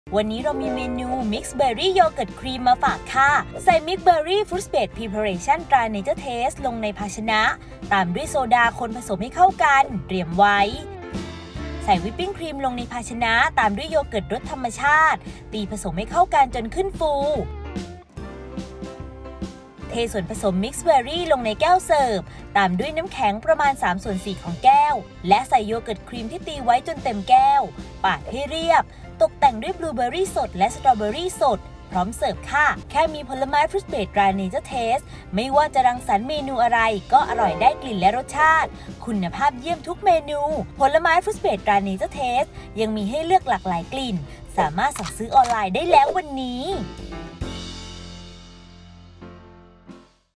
มิกซ์เบอร์รี่โยเกิร์ตครีม ทำง่าย สไตล์ "อ.ยิ่งศักดิ์"
มิกซ์เบอร์รี่โยเกิร์ตครีม  ผลิตภัฑณ์ เนเจอร์เทส

มิกซ์เบอร์รี่ ฟรุ๊ตเบสพรีเพอเรชั่น ตราเนเจอร์เทส 15  มิลลิลิตร
โซดา                  30 มิลลิลิตร
วิปปิงครีม                 90 มิลลิลิตร
โยเกิร์ตรสธรรมชาติ 30 กรัม
น้ำแข็งสำหรับรับประทานคู่
บลูเบอร์รีและสตรอว์เบอร์รี สำหรับตกแต่ง

1. ใส่มิกซ์เบอร์รี่ ฟรุ๊ตเบสพรีเพอเรชั่น ตราเนเจอร์เทส ลงในภาชนะตามด้วยโซดาคนผสมให้เข้ากันเตรียมไว้
2. ใส่วิปปิงครีมลงในภาชนะตามด้วยโยเกิร์ตรสธรรมชาติตีผสมให้เข้ากันจนขึ้นฟู
3. เทส่วนผสมมิกซ์เบอร์รี่ลงในแก้วเสิร์ฟตามด้วยน้ำแข็งประมาณ ¾ ของแก้ว และใส่โยเกิร์ตครีมที่ตีไว้จนเต็มแก้ว ปาดให้เรียบ ตกแต่งด้วยบลูเบอร์รีสดและสตรอว์เบอร์รีสด พร้อมเสิร์ฟ

▲Line @yingsakfood